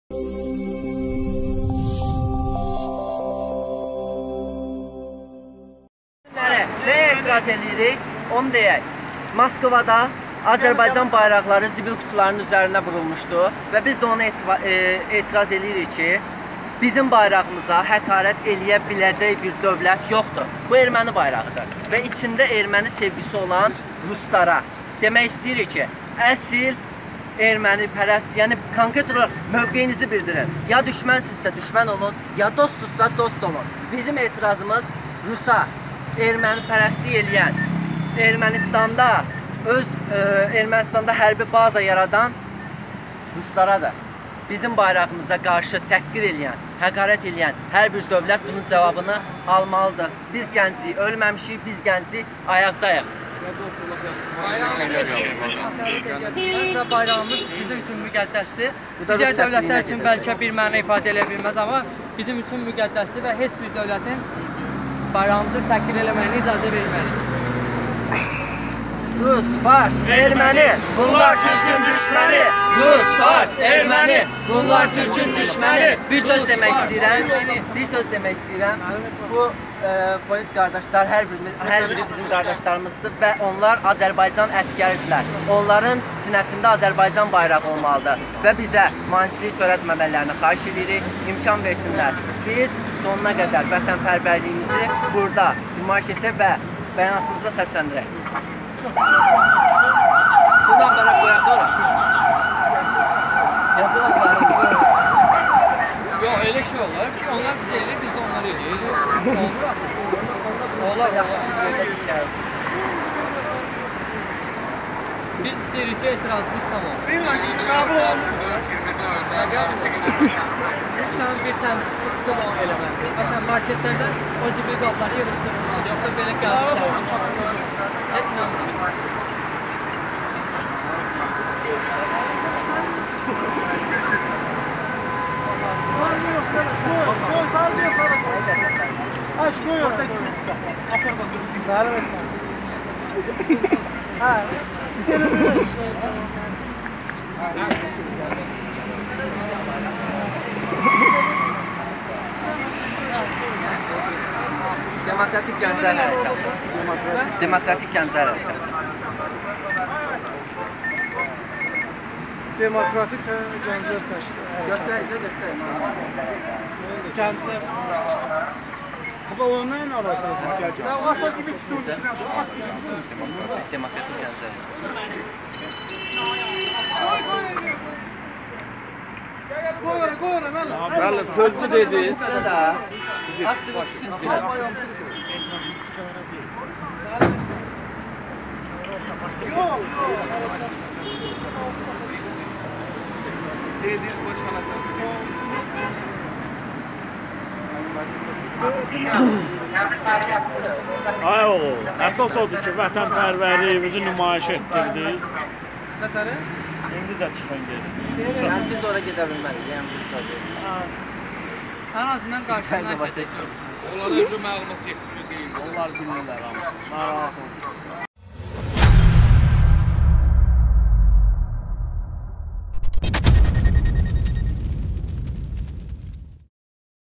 Azərbaycan bayrağına hörmətsizlik edən Rusiya səfirliyinin önündə aksiya (Kanala abunə olun)
"Bizim etirazımız ruslaradır - bayrağımızı təhqir edın hər kəs bunun cavabını almalıdır: rus, fars, erməni - bunlar türkün düşməni"...

Yanvarın 23-də Demokratik Gənclər Hərəkatının sədri və bir qrup şəxs Rusiyanın Bakıda səfirliyinin önündə etiraz aksiyası keçirib. Səbəb isə qısa müddət öncə Rusiyanın paytaxtı Moskva şəhərində zibil qutularının üstünə Azərbaycan bayrağı vurulması olub.